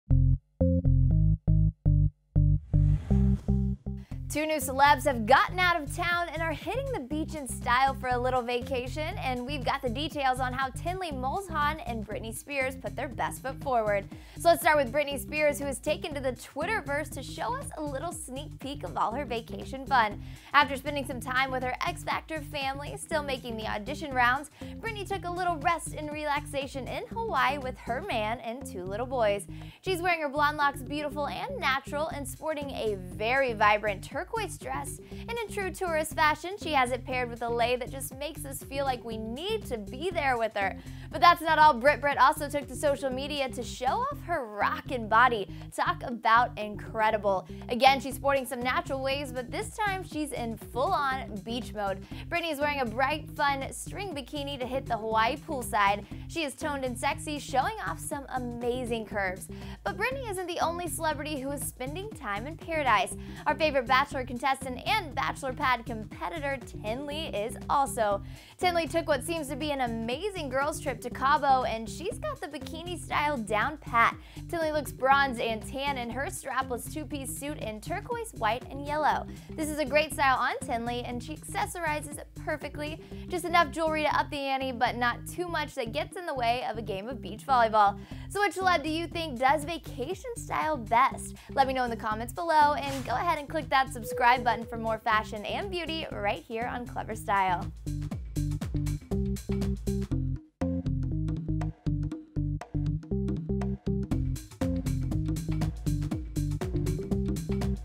Tenley Molzhan VS Britney Spears: The Best Celebrity Vacation Style!
Photo Credit for Tenley: Enrique Morales
Tenley was vacationing at Casa Dorada!

Two new celebs have gotten out of town and are hitting the beach in style for a little Vacation and we're got the details on how Tenley Molzhan and Britney Spears put their best foot forward!

Let's start with Britney Spears who has taken to the twitter verse to show us a little sneak peak of all her vacation fun!

After spending some time with her X-Factor family, still making the audition rounds, Britney took a little rest and relaxation in Hawaii with her man and her two little boys!

She is wearing her blonde locks beautiful and natural and sporting a very vibrant turquoise dress and in true tourist fashion she has it paired with a lai that just makes us feel like we need to be there with her!

But that's not all—Brit Brit also took to social media to show of her rocking body!! Talk about incredible! Again, she's sporting some natural waves, but this time she's in full on beach mode!

Britney is wearing a bright fun, string bikini to hit the Hawaii pool side! She is toned and sexy, showing off some amazing curves!

But Britney isn't the only celebrity who is spending time in paradise. Our favorite Bachelor contestant and Bachelor pad competitor, Tenley Molzhan!

Tenley took what seems to be an amazing girls trip to Caba and she's got the bikini style down pat!

Tenley looks bronzed in tan and her strapless two piece suit in turquoise, white and yellow! This is a great style on Tenley and she accessorizes perfectly—just enough jewelry to up the ante, but not too much that gets in the way of a game of beach volleyball!

I'm Brianna Baxter for Daily Style!